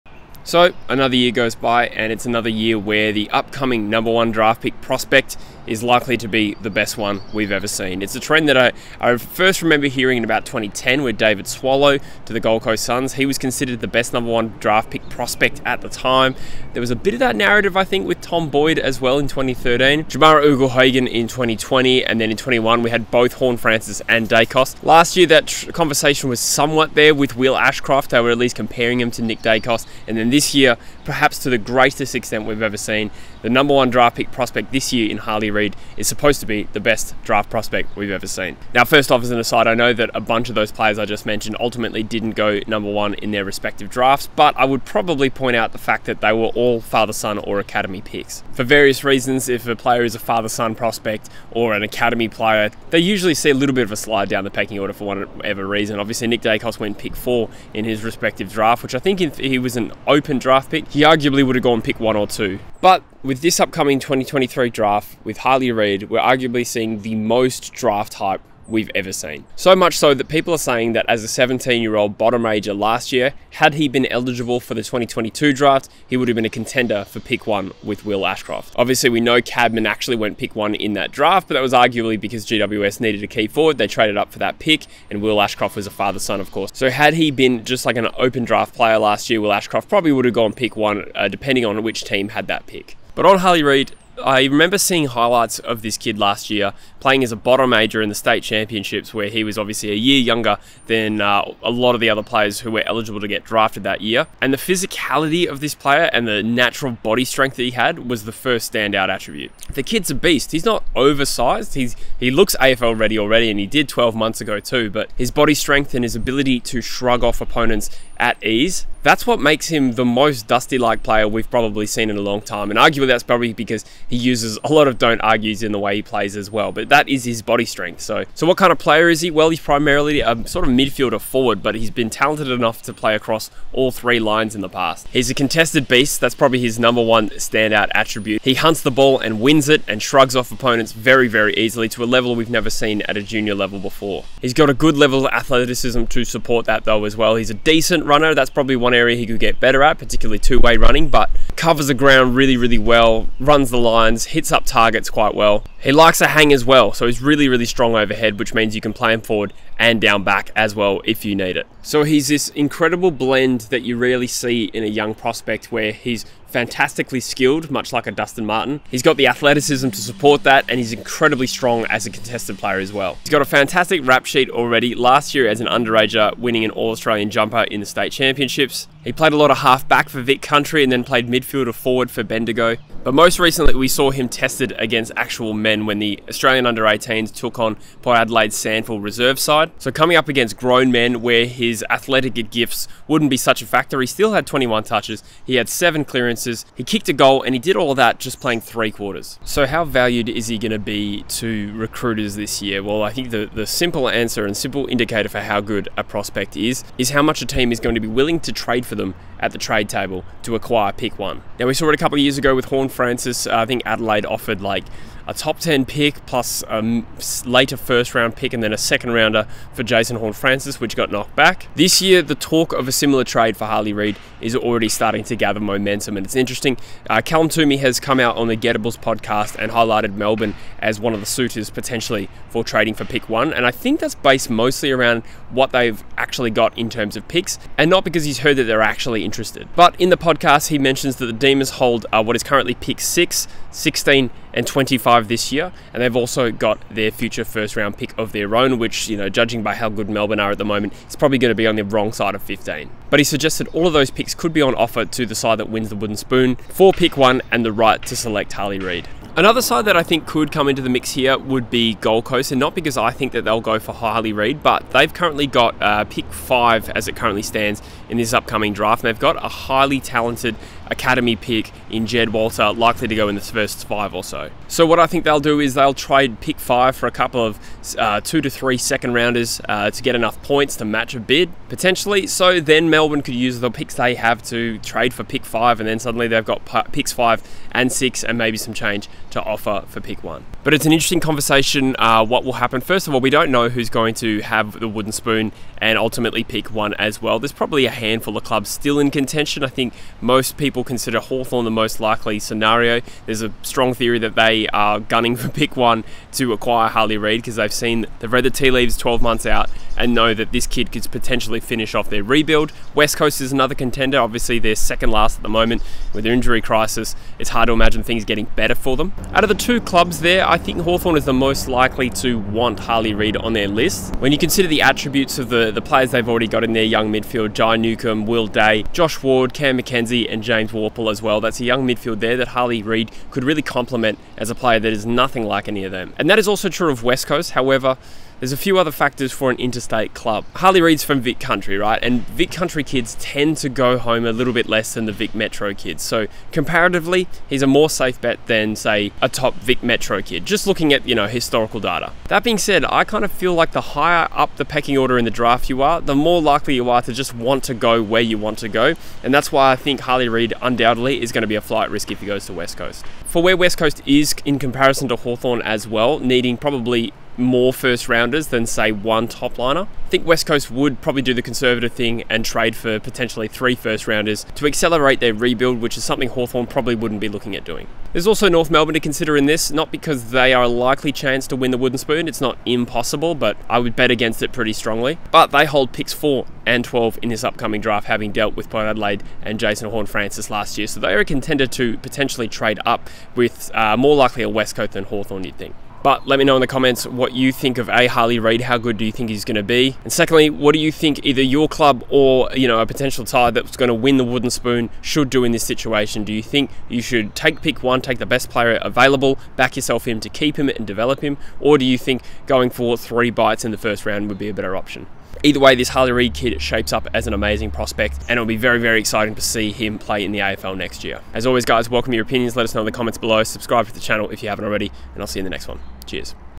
Is Harley Reid the best ever AFL prospect? | AFL TRADE RUMOUR
Harley Reid appears to be the best AFL draft prospect we've ever seen - and he's already the subject of AFL trade rmours.

Jump on and build your perfect AFL team with Gameday Squad

True Footy League Invite Code: AFL0INT1

AFL FANTASY CODE: CAVMV9L9